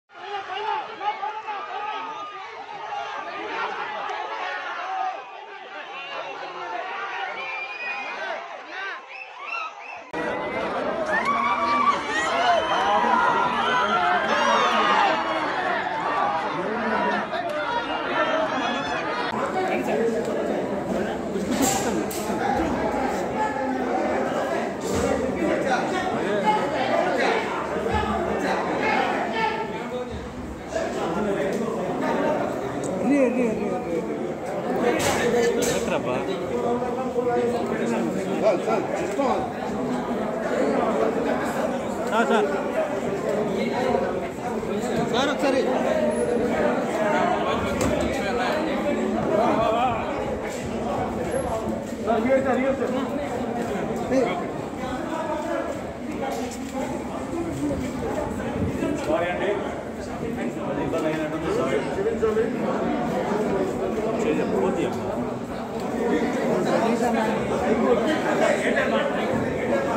Pawan Kalyan Submitts Application to Collectarate to Help Farmers || Janasena Party || CC
Watch Here  Pawan Kalyan Submitts Application to Collectarate to Help Farmers